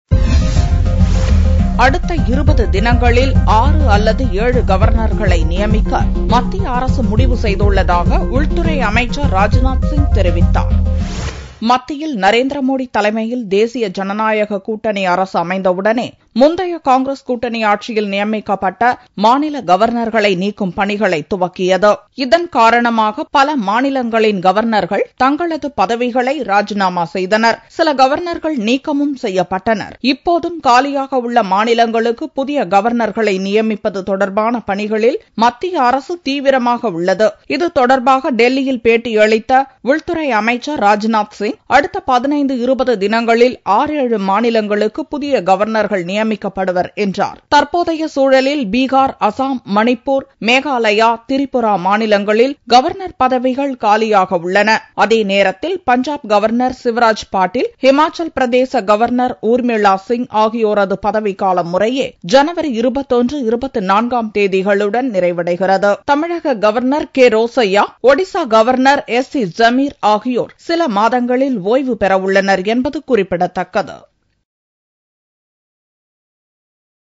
Government plans to appoint 6-7 Governors in coming weeks: Rajnath Singh
Government plans to appoint 6-7 Governors in coming weeks: Rajnath Singh - Dinamalar Jan 18th 2015 Tamil Video News For more videos